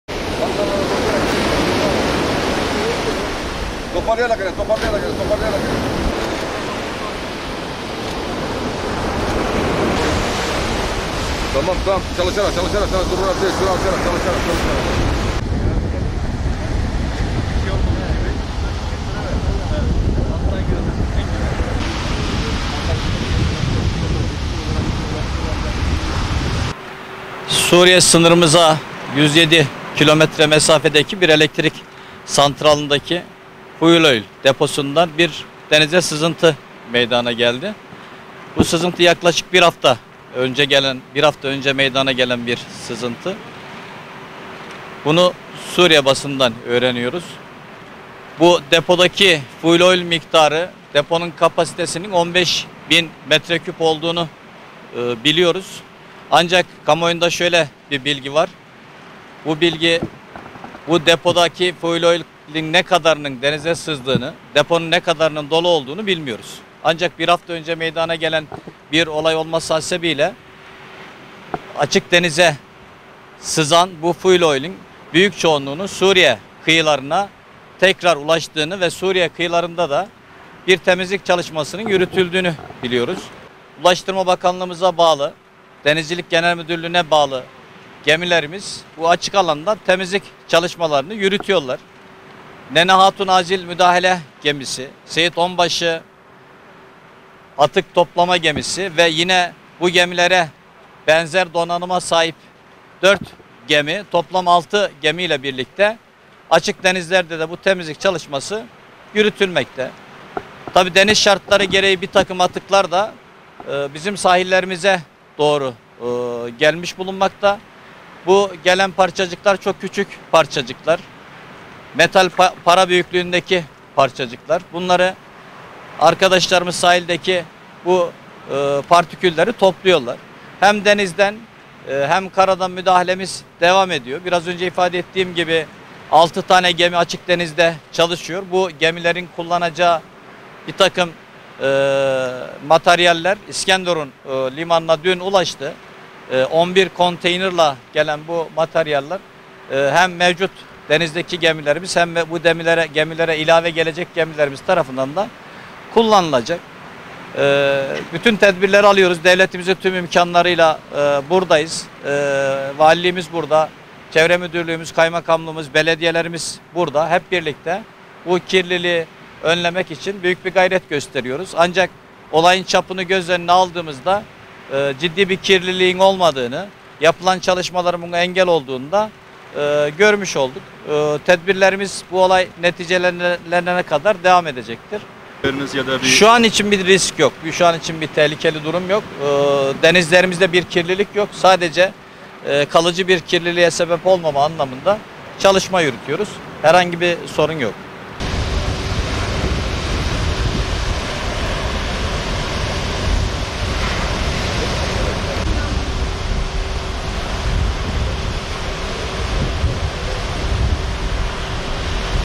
HBB kıyılara ulaşan akaryakıt sızıntısına müdahale ediyor
Hatay’ın mavi sularının temizliği için çalışma başlatıldı
Hatay Büyükşehir Belediyesi(HBB) Suriye’de bulunan elektrik santralindeki akaryakıt deposunda oluşan sızıntı sebebiyle kıyılarda oluşan kirliliği ortadan kaldırmak için kolları sıvadı.
Suriye'de bir enerji santralinden yayılan petrol sızıntısının Hatay sularına ulaştığının fark edilmesi üzerine Ulaştırma Bakanlığı ve Denizcilik Genel Müdürlüğü denizlerde kapsamlı bir temizlik çalışması başlattı.
HBB Çevre Koruma ve Kontrol Daire Başkanlığı da kıyılara ulaşan sızıntının kalıcı kirliliğe yol açmaması için çalışma başlattı.
Konu ile ilgili açıklamalarda bulunan Hatay Valisi Rahmi Doğan, sızıntının kalıcı bir kirliliğe dönüşmemesi için 6 gemi ile açık denizde müdahale edildiğini ifade ederek, “Suriye sınırımıza 107 kilometrelik bir mesafedeki bir elektrik santralinin fuel oil deposundan denize bir sızıntı meydana geldi. Nene Hatun acil müdahale gemisi, Seyit Onbaşı atık toplama gemisi ve bu gemilere benzer donanıma sahip 4 gemi toplam 6 gemi ile birlikte açık denizde bu temizlik çalışması yürütülmekte” dedi.
HBB Ekipleri Vakit Kaybetmeden Temizlik Çalışmalarına Başladı
HBB Çevre Koruma ve Kontrol Daire Başkanlığı ekipleri de kıyıya ulaşan sızıntının bertaraf edilmesi için çalışmalara vakit kaybetmeden başladı. Büyükşehir ekipleri, kıyıya ulaşan petrol kalıntılarını temizlemek için 35 kişilik personeli ve çeşitli ekipmanlarıyla çalışma gerçekleştiriyor.
HBB yetkilileri, sorun ortadan kalkıncaya kadar temizlik çalışmalarının devam edeceğini duyurdu.